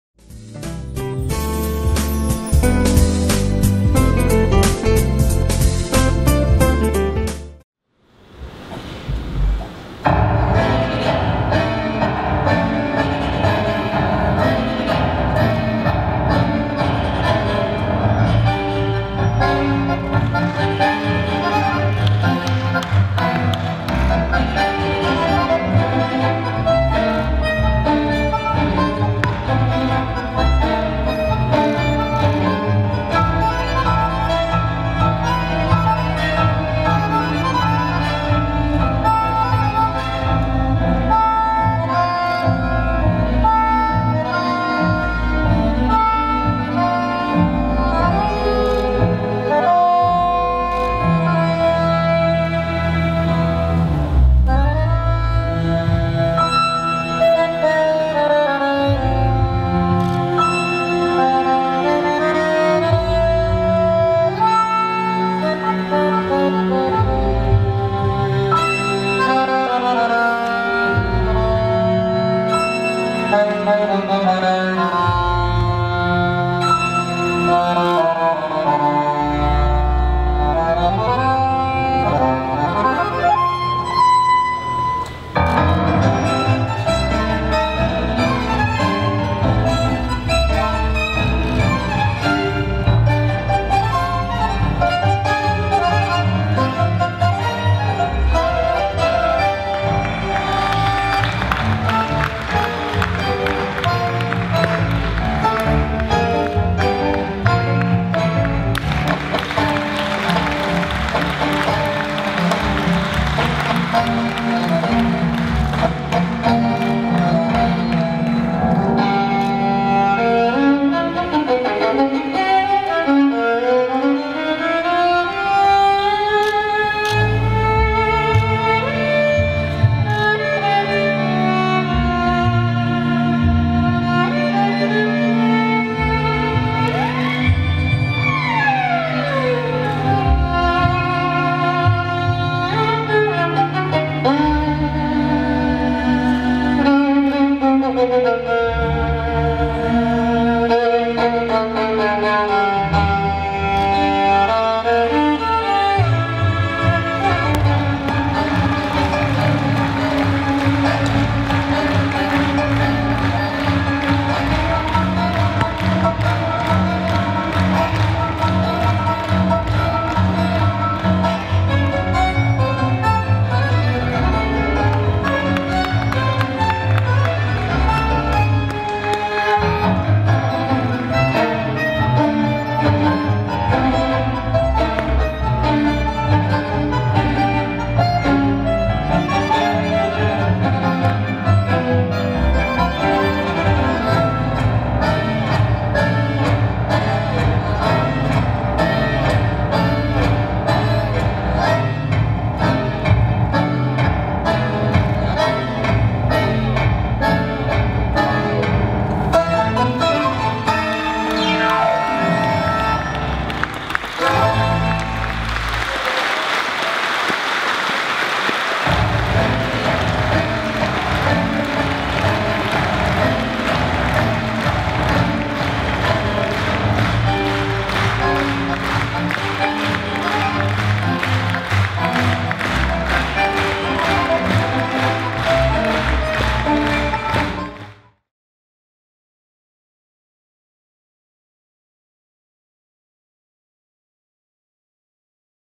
Welttanz-Gala 2017 Baden-Baden | Pavel Zvychaynyy & Oxana Lebedew | Paso Doble
Pavel Zvychaynyy & Oxana Lebedew - Paso Doble at Welttanz-Gala 2017, Kurhaus Baden-Baden (4. November).